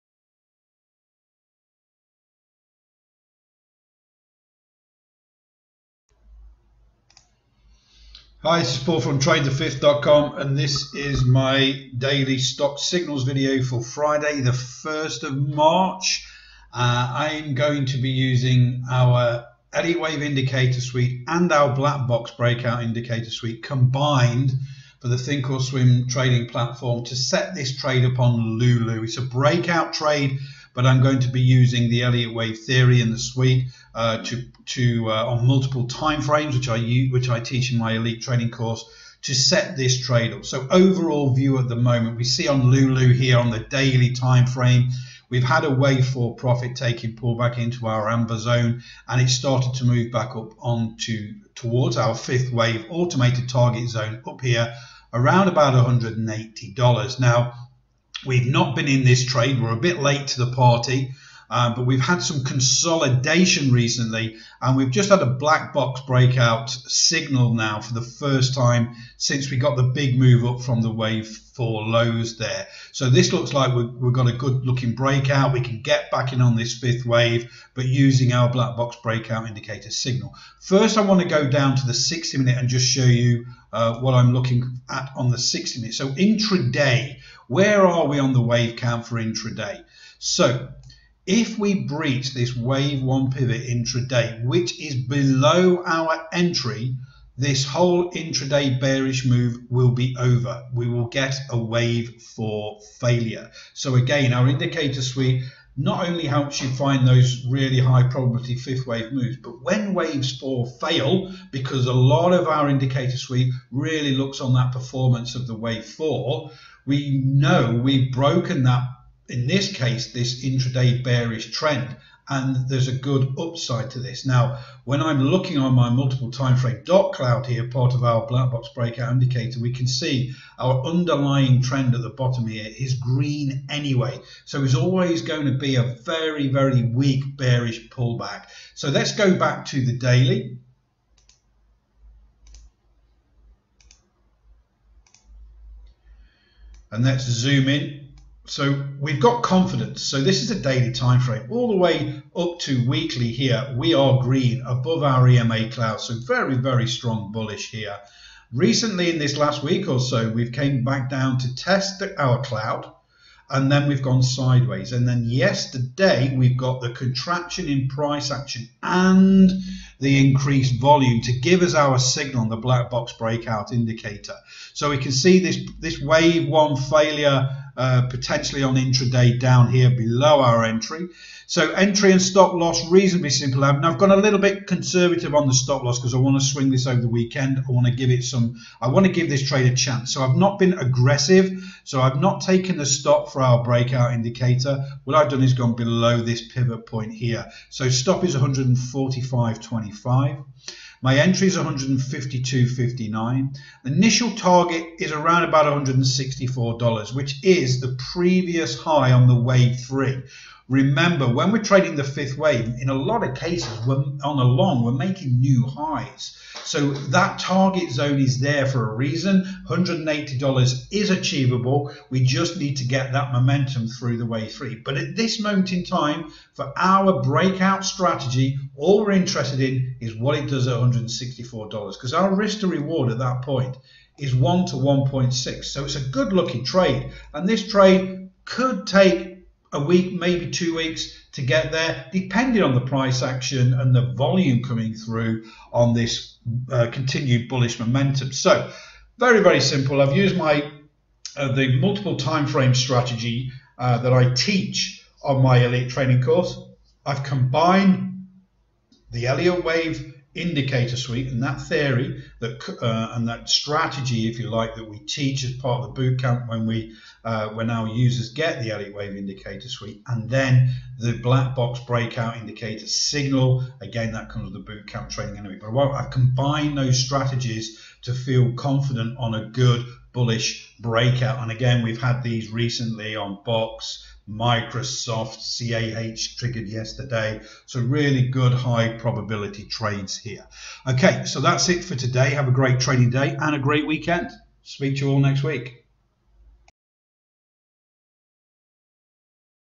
TradeTheFifth Stocks Signals Video for potential Long Breakout on LULU
Watch the Stocks Swing Trading Setup video for a potential long trade on LULU using the Elliott Wave Indicator Suite

The indicator Suite is available for NinjaTrader, TradeStation, ThinkorSwim and MultiCharts trading patforms.

 daily
stock signals video for Friday the first
of March I'm going to be using our
anyway Vindicator suite and our black
box breakout indicator suite combined
for the thinkorswim trading platform to
set this preyed upon
Lulu it's a breakout trade but I'm going
to be using the Elliot wave theory in
the suite to multiple time frames which
are you which I teach in my elite
training course to set this trade up so
overall view at the moment we see on
Lulu here on the daily timeframe we've
had a way for profit taking pull back
into our amber zone and he started to
move back up on to towards our fifth
wave automated target zone up here
around about a hundred and eighty
dollars now we've not been in this trade
we're a bit late to the party but we've
had some consolidation recently and
we've just had a black box breakout
signal now for the first time since we
got the big move up from the wave for
lows there so this looks like we're
going to good looking breakout we can
get back in on this fifth wave but using
our black box breakout indicator signal
first I want to go down to the 60 minute
and just show you what I'm looking at on
the 60 Minutes so intrigued a where are
we on the wave cam for intraday so if we
breach this wave one pivot intraday
which is below our entry this whole
intraday bearish move will be over we
will get a wave for failure so again our
indicator suite not only helps you find
those really high probability 5th wave
moves but when waves or fail because a
lot of our indicator sweep really looks
on that performance of the way for we
know we broken that in this case this
intraday bearish trend and there's a
good upside to this now when I'm looking
on my multiple time for a dark cloudy a
part of our black box breakout indicator
we can see our underlying trend at the
bottom here is green anyway so is always
going to be a very very weak bearish
pullback so they
go back to the daily and that's zoom in
so we've got confidence so this is a
daily timeframe all the way up to weekly
here we are green above area mail outs a
very very strong bullish here recently
in this last week or so we've came back
down to test the our cloud and then
we've gone sideways and then yesterday
we've got the contraction in price
action and the increased volume to give
us our signal the black box breakout
indicator so we can see this this way
one failure potentially on intraday down
here below our entry so entry and
stop-loss reasonably simple and I've
gone a little bit conservative on the
stop-loss because I want to swing this
over the weekend if I want to give it
some I want to give this trade a chance
so I've not been aggressive so I've not
taken the stock four-hour break our
indicator what I've done is gone below
this pivot point here so stop is 140 525
my entries 150 259 initial target is
around about 164 dollars which is the
previous high on the wave 3 remember
when we're trading the fifth wave in a
lot of cases on the lawn were making new
highs
so that target zone is there for a
reason one hundred and eighty dollars is
achievable we just need to get that
momentum through the way through but at
this moment in time for our breakout
strategy all we're interested in is what
if there's a hundred and sixty four
dollars because our risk to reward at
that point is one to one point six so
it's a good-looking trade and this trade
could take a week maybe two weeks to get
there depending on the price action and
the volume coming through on this
continued bullish momentum so very very
simple I've used my the multiple
timeframes strategy that I teach on my
elite training course I've combined the
Elliott Wave indicator suite and that
theory the and that strategy if you like
that we teach is part of the bootcamp
when we when our users get the Elliott
Wave indicator suite and
the black box breakout indicators signal
again that kind of the bootcamp training
I combine those strategies to feel
confident on a good bullish breakout and
again we've had these recently on box
Microsoft CAH triggered yesterday so
really good high probability trades here
ok so that's it for today have a great
training day and a great weekend speech
you all next week